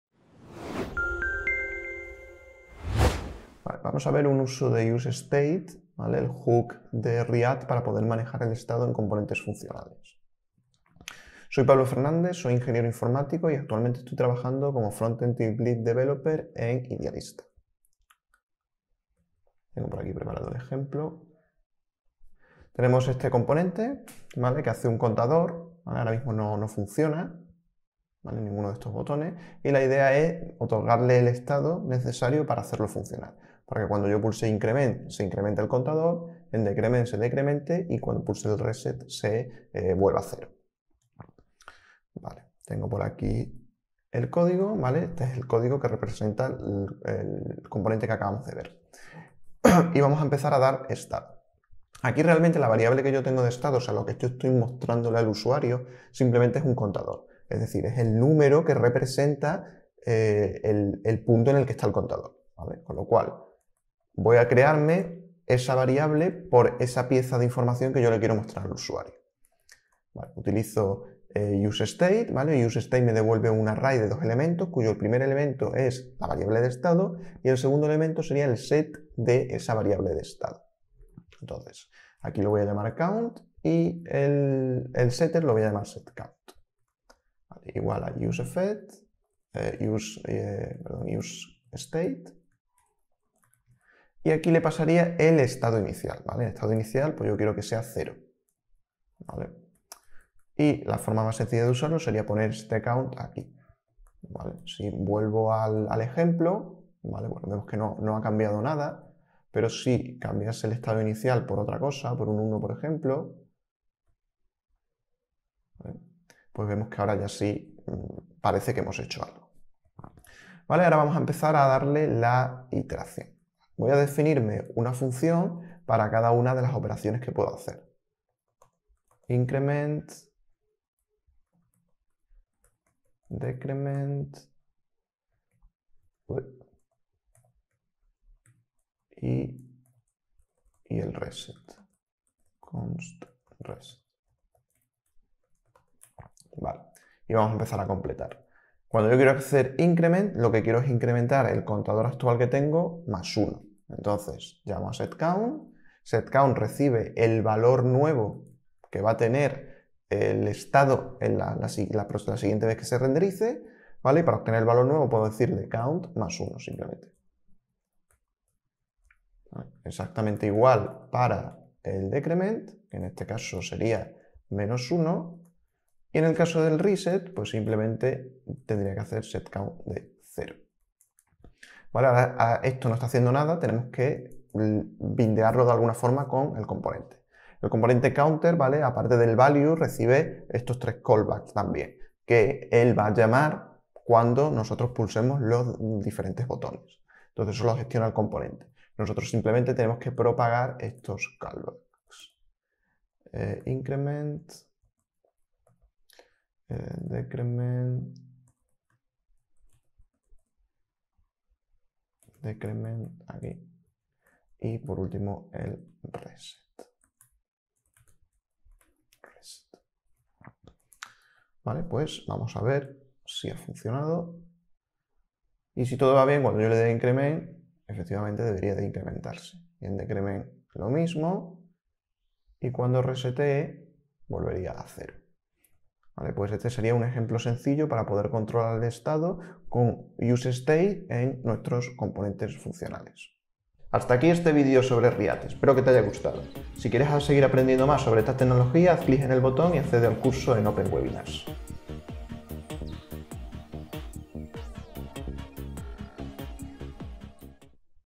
Uso de useState en React
Si quieres aprender a utilizar useState, un Hook de React para manejar el estado de componentes funcionales, no dejes pasar este video en el que te lo mostramos de forma práctica.